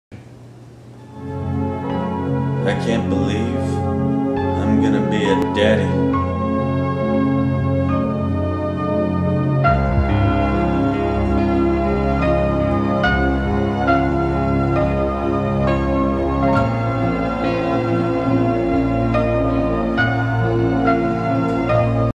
Over Dramatic 101
Watch as Mr. X ponders the meaning of life with the realization that he is going to be a father.